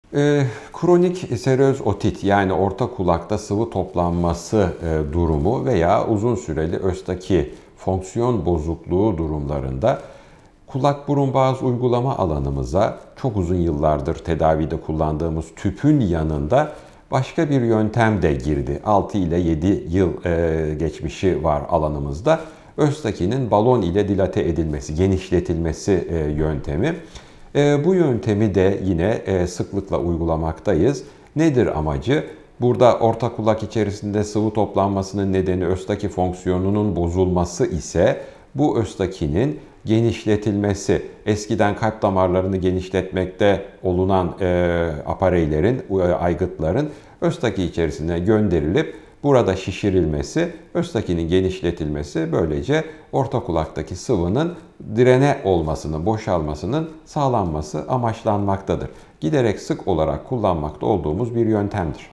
Orta Kulakta Sıvı Toplanması - Östaki Balon Genişletme - Prof. Dr. Haluk Özkarakaş
Kronik seröz otit, yani orta kulakta sıvı birikmesi ya da uzun süreli östaki fonksiyon bozukluğu durumlarında, yıllardır tedavide kullandığımız kulağa tüp takma yöntemine ek olarak yeni bir yöntem daha uygulanmaya başlandı. Bu yöntemin yaklaşık 6-7 yıllık bir geçmişi var. Östakinin balon ile genişletilmesi (dilatasyonu) olarak bilinen bu yöntemi sıklıkla kullanıyoruz.

Peki, bu yöntemin amacı nedir? Eğer orta kulakta sıvı birikmesinin nedeni östaki tüpünün düzgün çalışmamasıysa, balon ile östakiyi genişletmek bu sorunu çözebilir. Bu işlem, eskiden kalp damarlarını genişletmek için kullanılan cihazların östaki tüpüne yerleştirilmesi ve şişirilmesi yoluyla yapılır. Böylece östaki genişler ve orta kulakta biriken sıvının boşaltılması sağlanır.

Giderek daha sık uyguladığımız etkili bir yöntemdir.